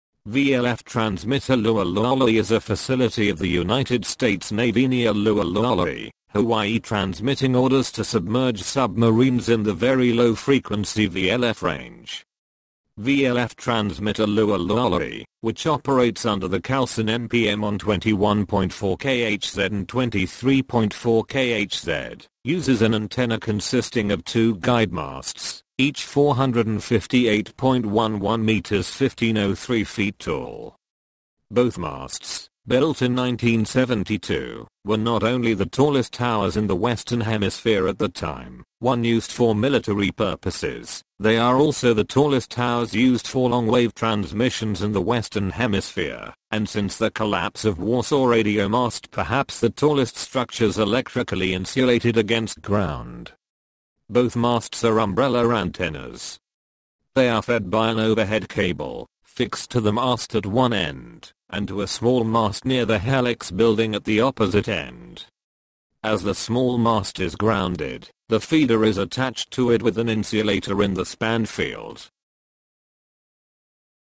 Lualualei VLF transmitter
VLF transmitter Lualualei is a facility of the United States Navy near Lualualei, Hawaii transmitting orders to submerged submarines in the very low frequency (VLF) range. VLF transmitter Lualualei, which operates under the callsign NPM on 21.4 kHz and 23.4 kHz, uses an antenna consisting of two guyed masts, each 458.11 metres (1503 feet) tall. Both masts, built in 1972, were not only the tallest towers in the Western hemisphere at the time,[1] used for military purposes, they are also the tallest towers used for long wave transmissions in the Western hemisphere and since the collapse of Warsaw Radio Mast perhaps the tallest structures electrically insulated against ground.

Both masts are umbrella antennas. They are fed by an overhead cable, fixed to the mast at one end, and to a small mast near the helix building at the opposite end. As the small mast is grounded, the feeder is attached to it with an insulator in the span field.